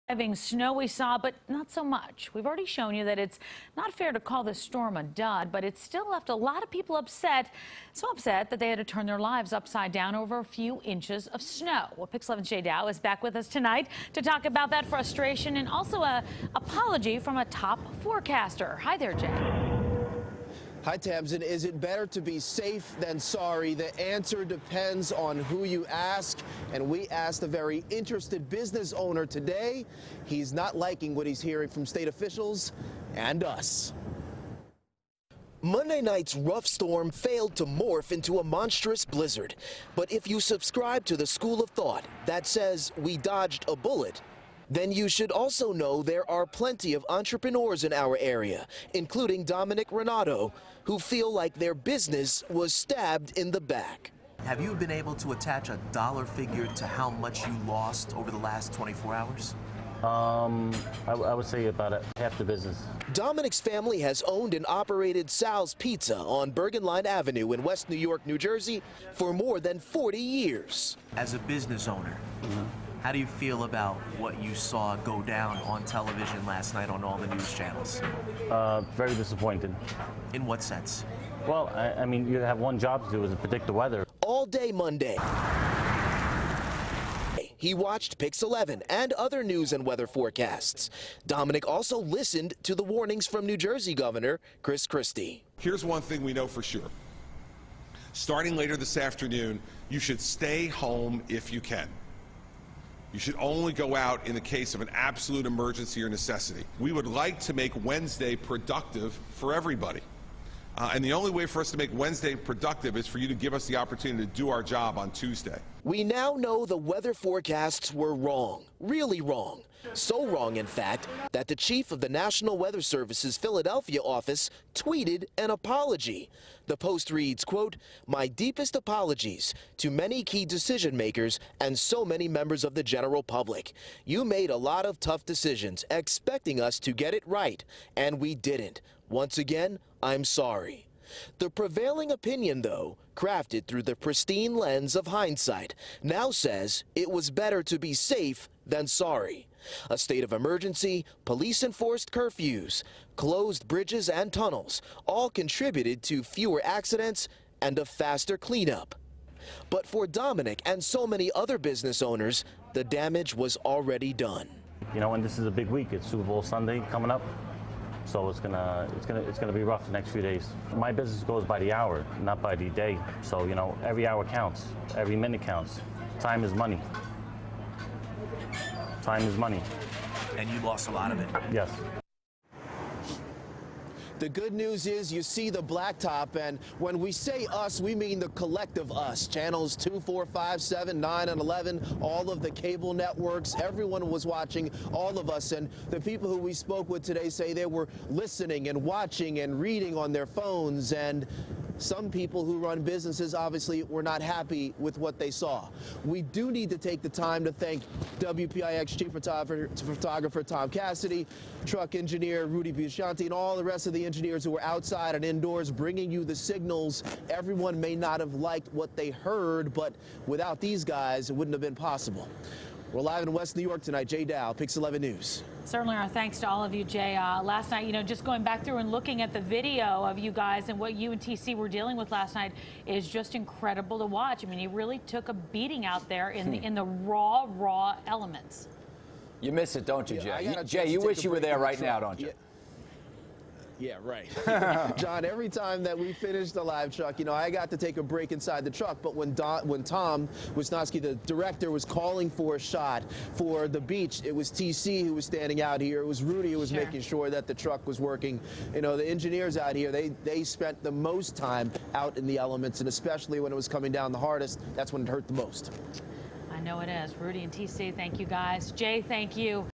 Jay Dow: "Blizzard Juno - Better Safe Than Sorry?"
We (the media) & elected officials across our area told Dominic the storm was going to be craaaaaazzzzzyy. So he closed his West New York, NJ restaurant early. Now, he and countless other business owners are paying the price. You can watch Jay Dow live on the Pix11 News @ 5pm and 10pm and be sure to log on to pix11.com.